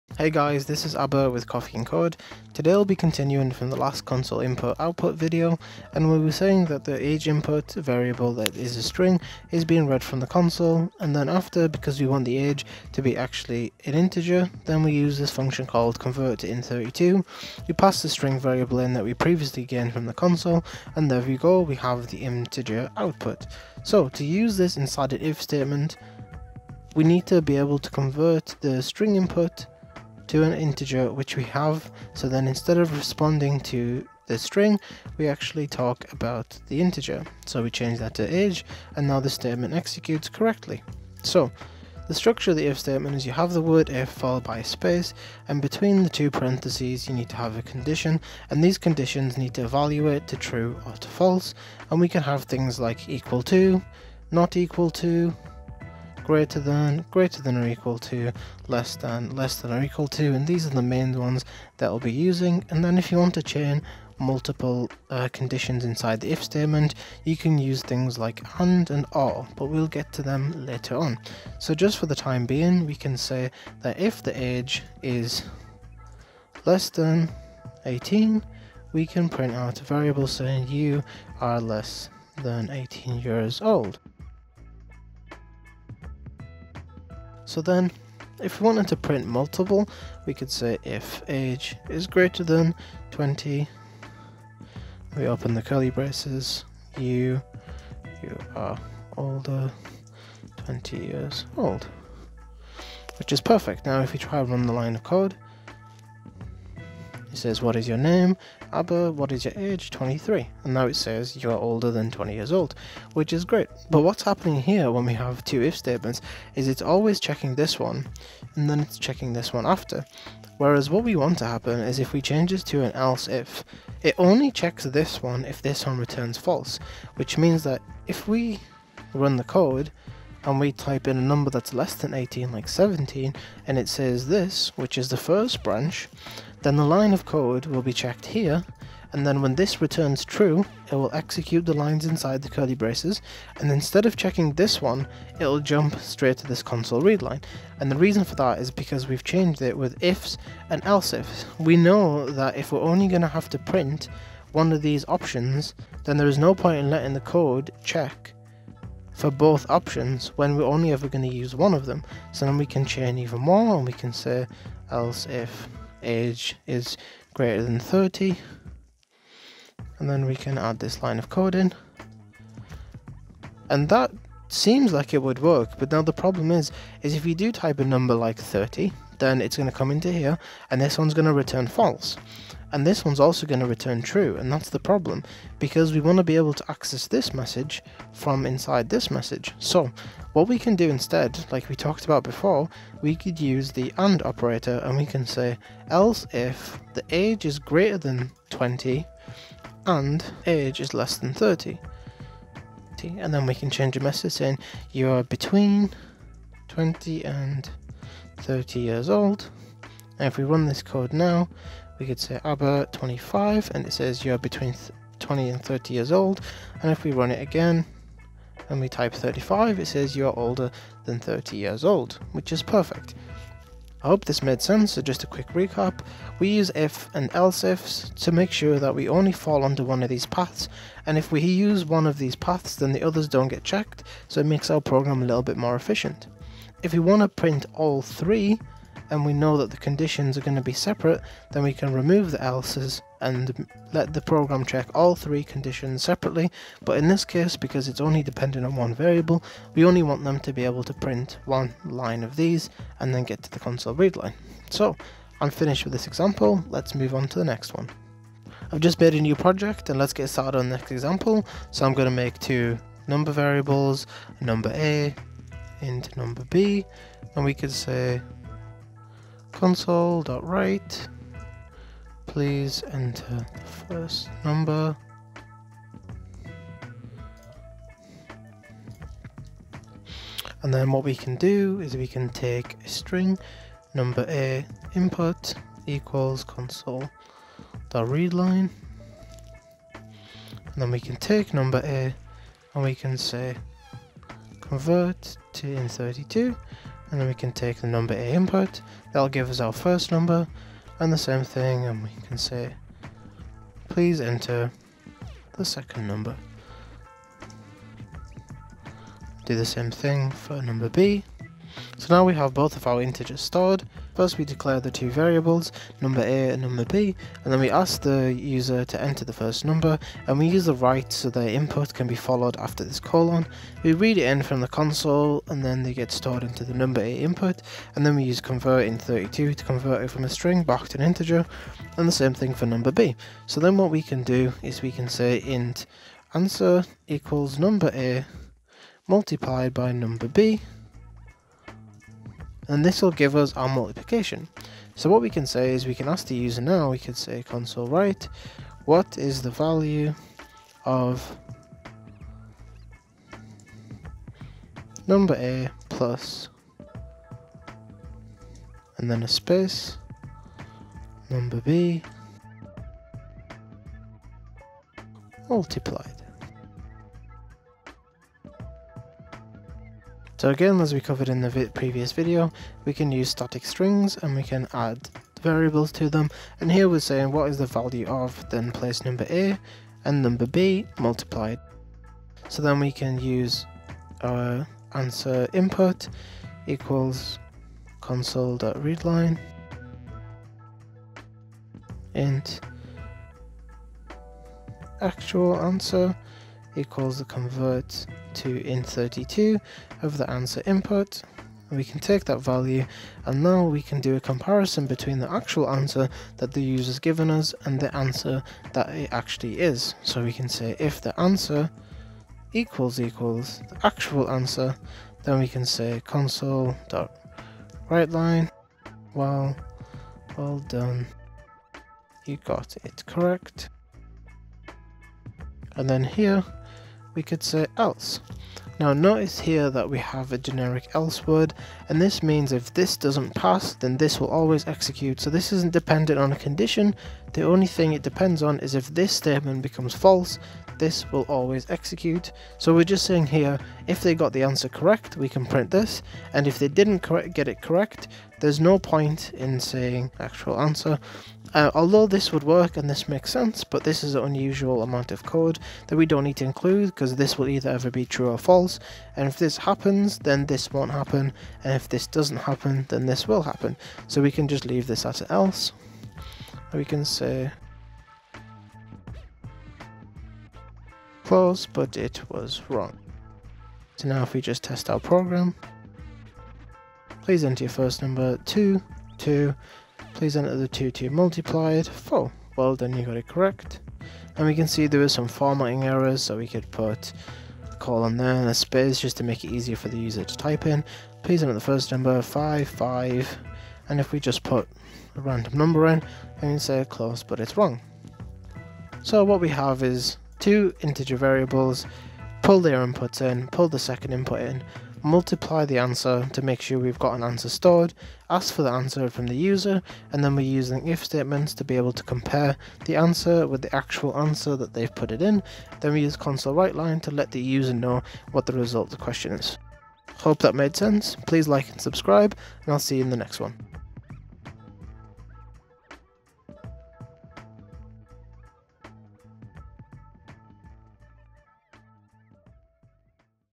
#3 If Statements - C# Tutorials for Beginners | CoffeeNCode
This video takes you through the process to learn and understand how to integrate if statements with console input to create a program with different output paths.

When you join the server, you can get your own personal help channel for FREE where you can post anything you're stuck with and I will help you 😊